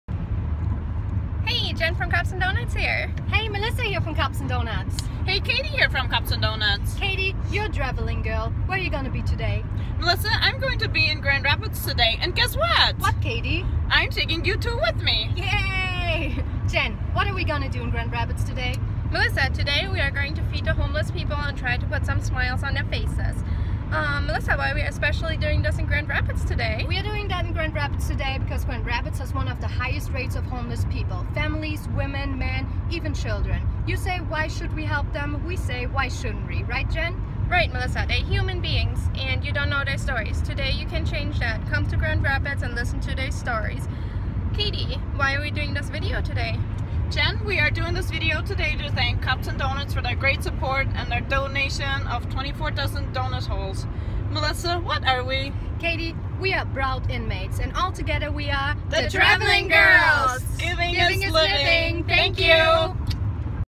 Cops & Doughnuts "Traveling Girls"
We are so proud of our Cops & Doughnuts "traveling girls" headed to Grand Rapids, Michigan to help feed the homeless !!